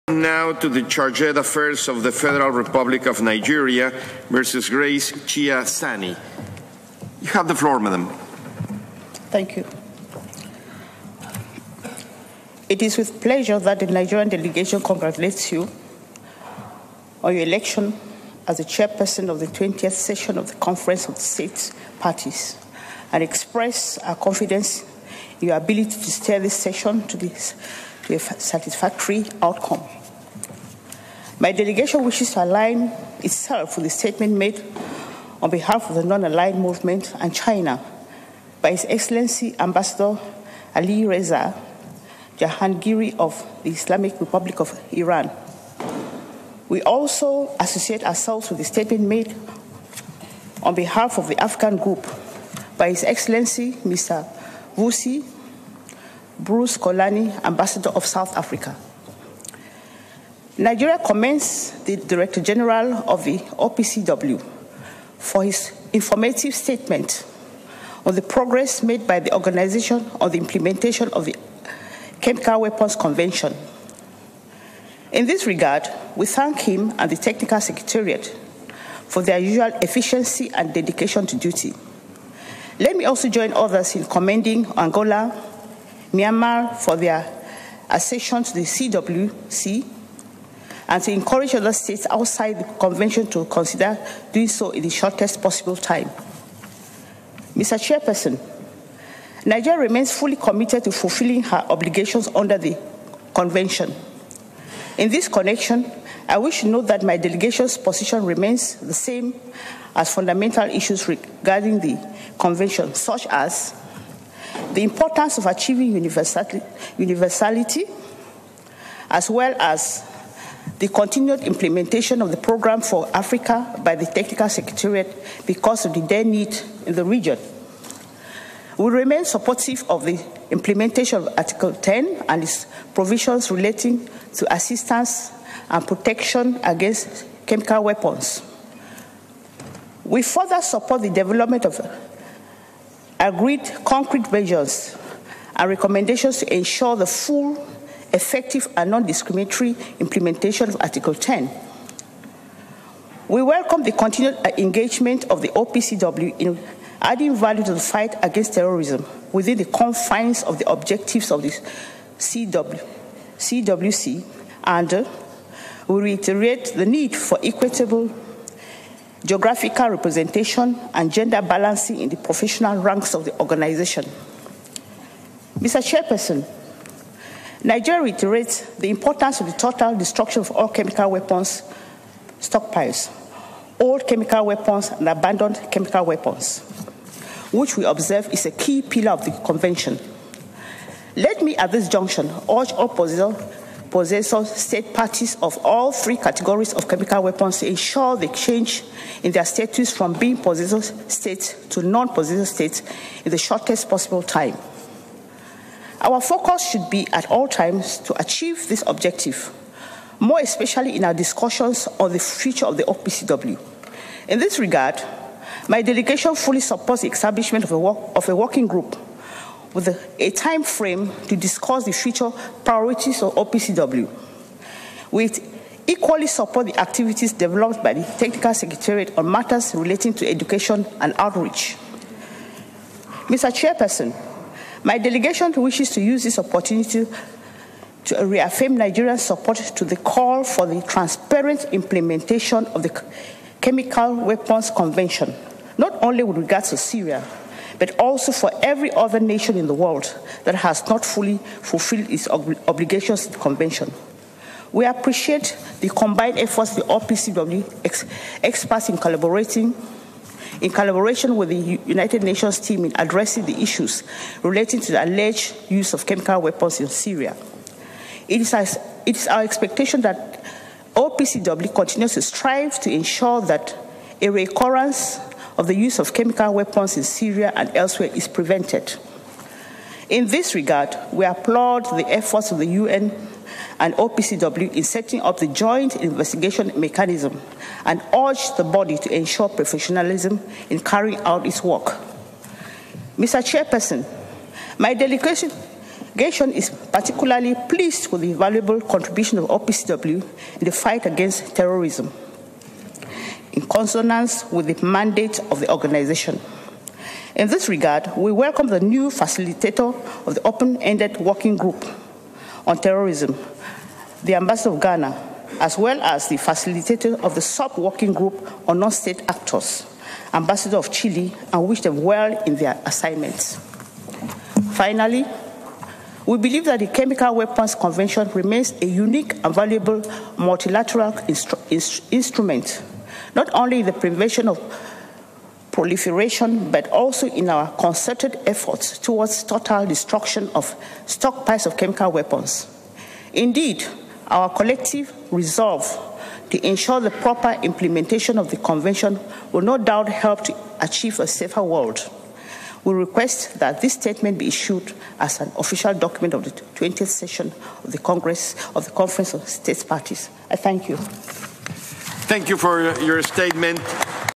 Nigeria Statement to the CSP-20 delivered by Mrs Grace Chia-Sani
Statement by Mrs Grace Chia-Sani at the 20th session of the Conference of States Parties to the Chemical Weapons Convention. Held at the World Forum Conference Centre the Hague, Netherlands. 2 December 2015.